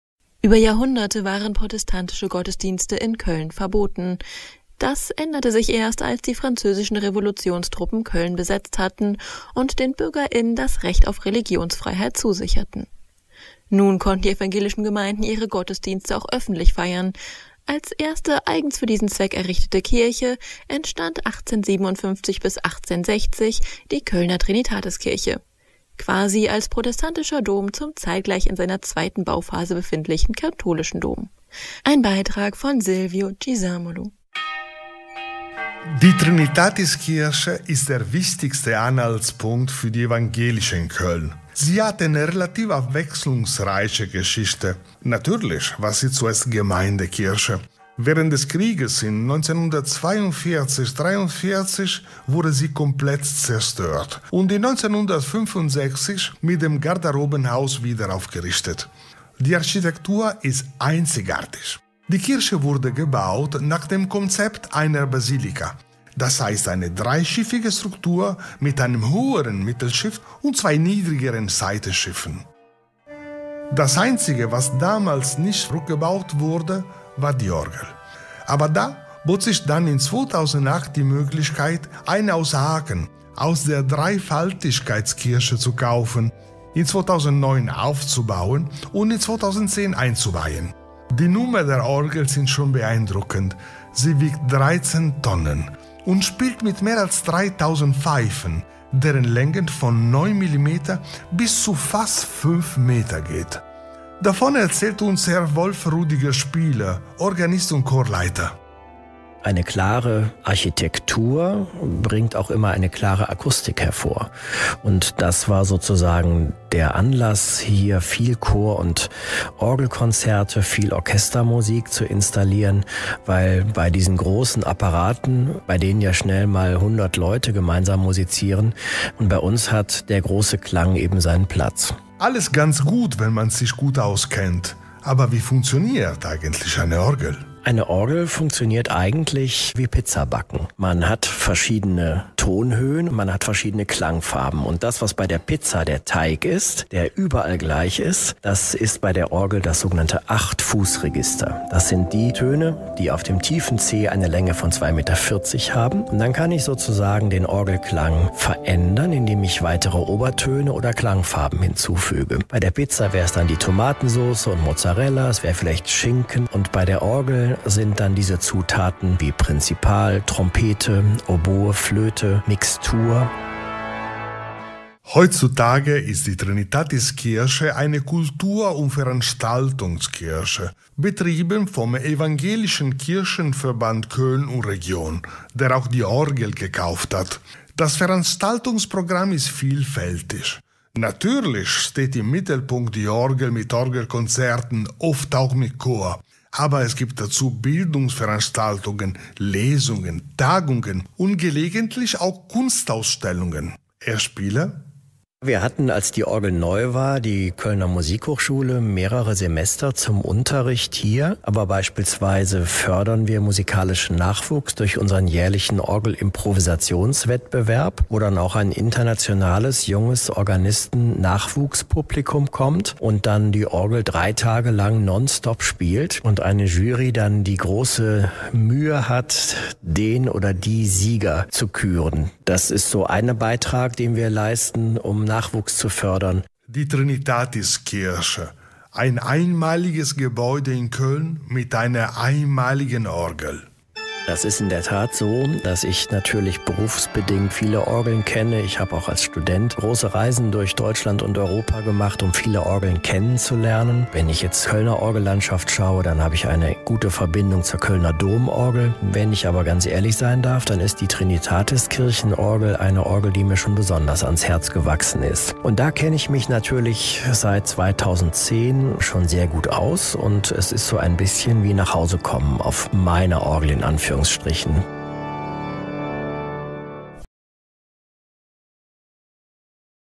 Radio Köln: Die Trinitatiskirche und ihre Orgel 25.11.21 Silvio Cisamolo
Die Trinitatiskirche in Köln und ihre Orgel
von Silvio Cisamolo
gesendet von Radio Köln am 25.11.21